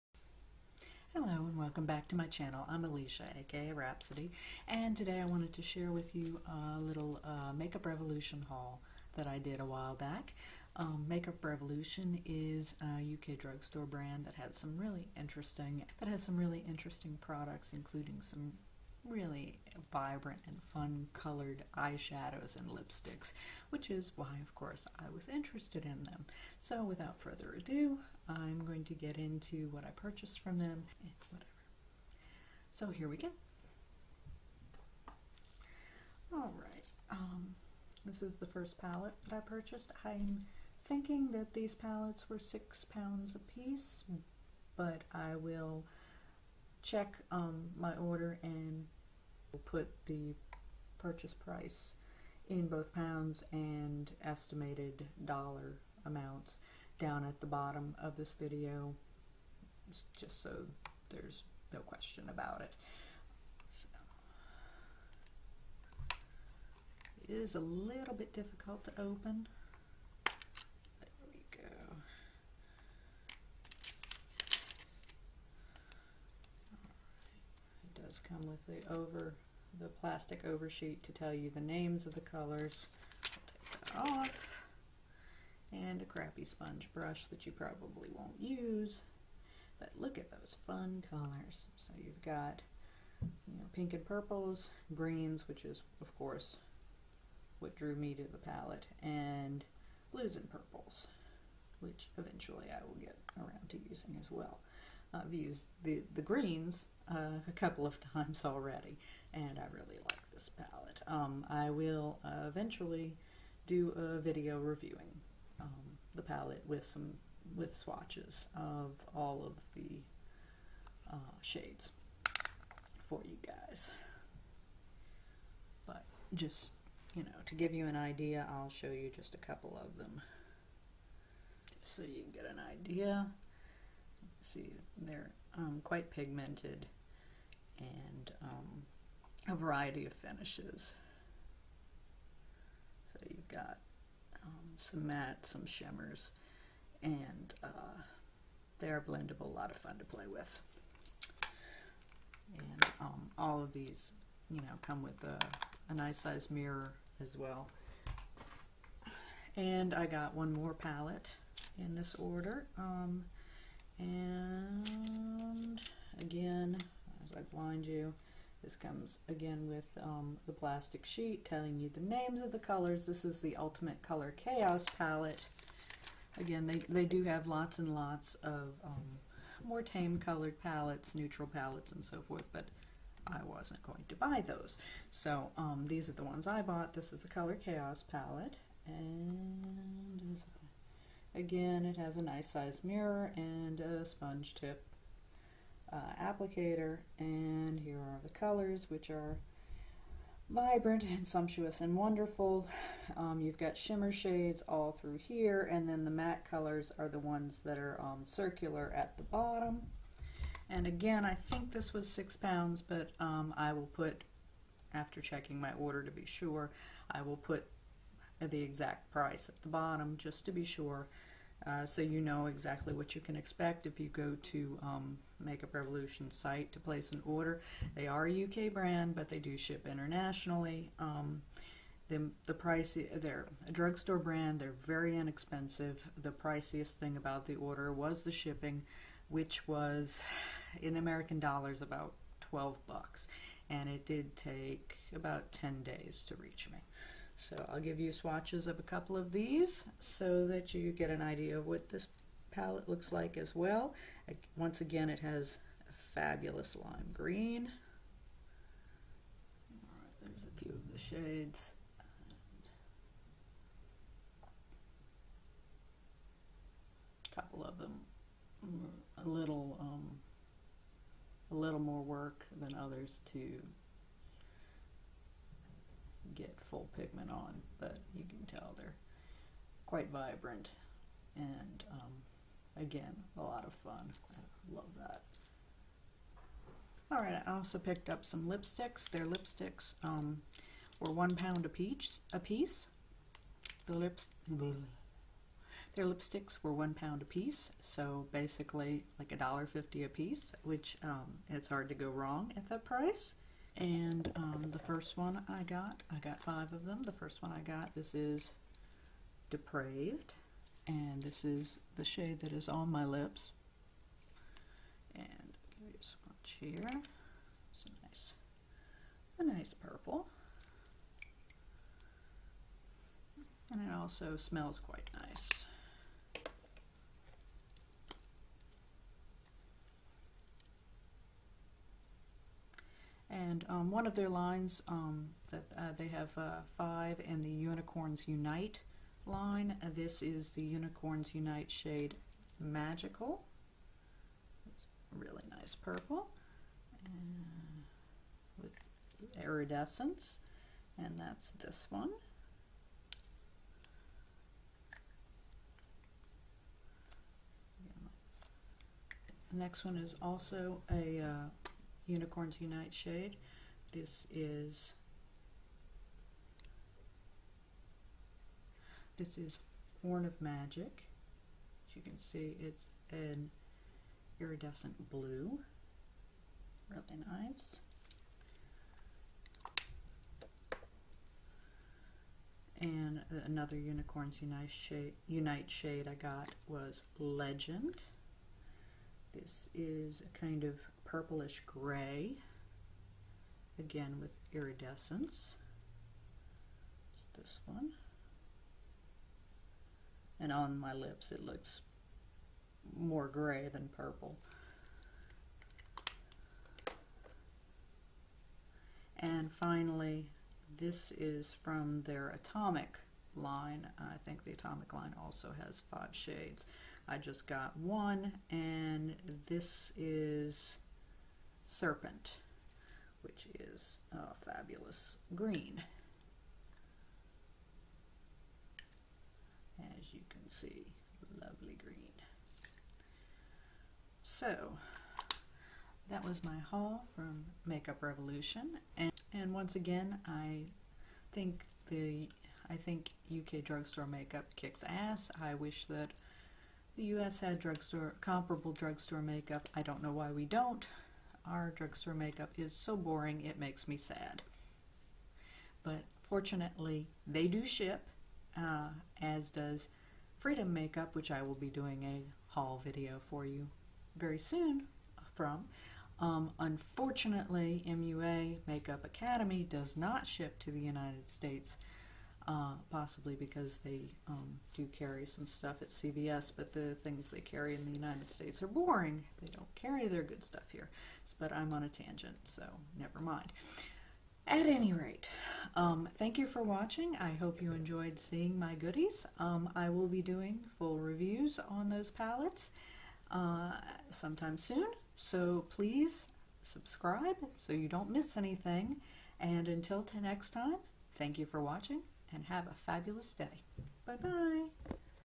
Makeup Revolution - Cosmetics Haul (with Swatches)
Look below for all the info! :)

This is a haul video of products I purchased from Makeup Revolution's website.
I purchased all of these items with my own money.